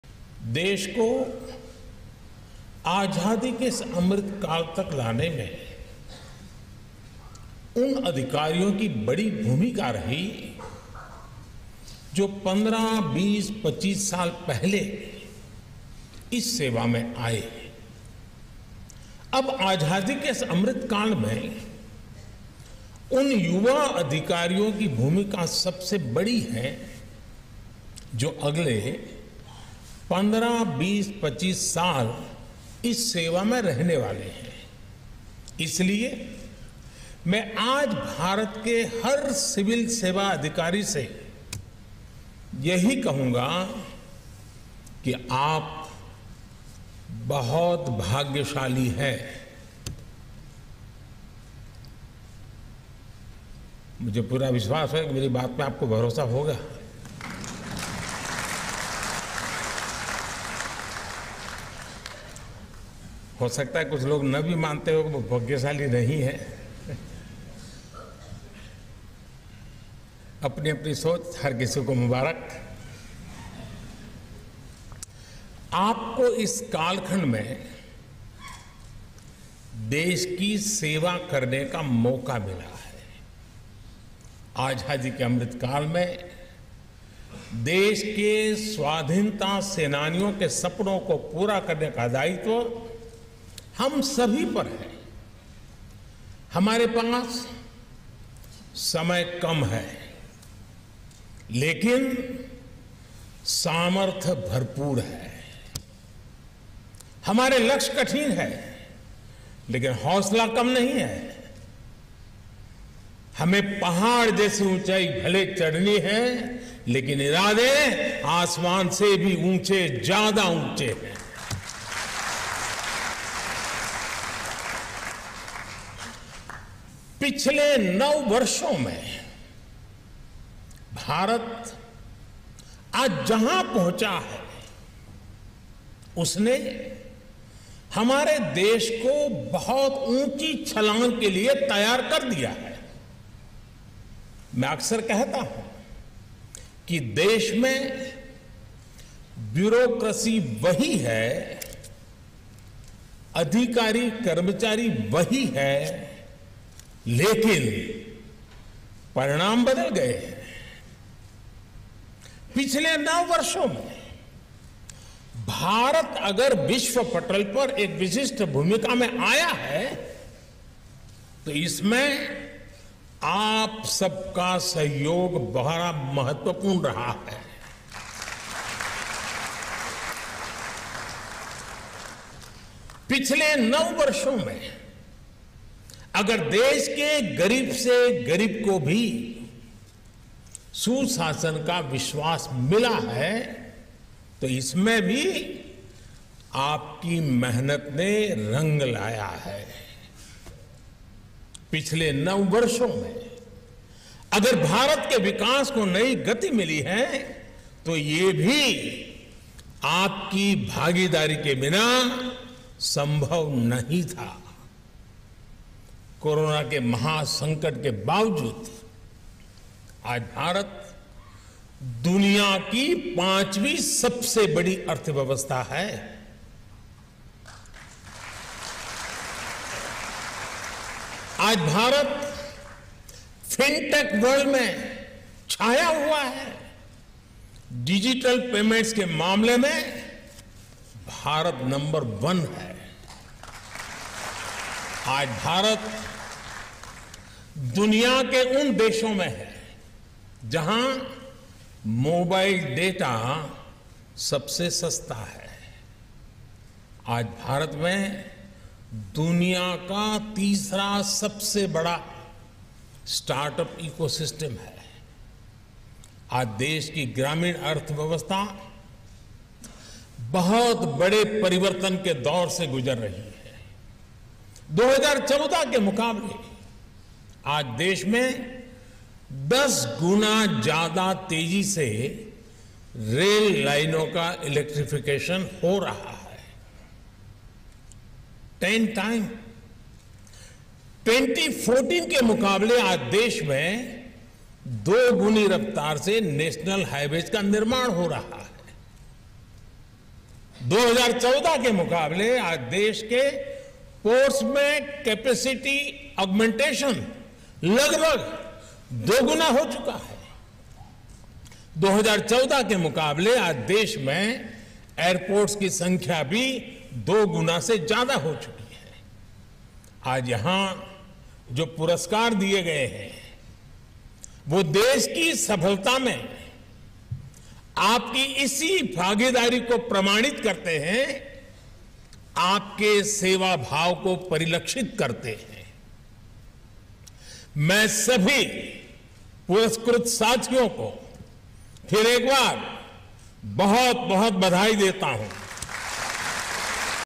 मोदी राज में चमत्कार! देश में Bureaucracy वही, लेकिन परिणाम बदल गए!
PM Modi addressed Civil Servants on Civil Services Day, 2023. He highlighted the contributions of Civil Servants who joined the service 15-25 years ago and emphasized the role of young officers who will contribute towards nation-building in the next 25 years of Amrit Kaal. PM Modi acknowledged the role of Karamyogis for the growing confidence of the poorest of the poor in ‘Sushasan’ and for a new momentum of development of the country.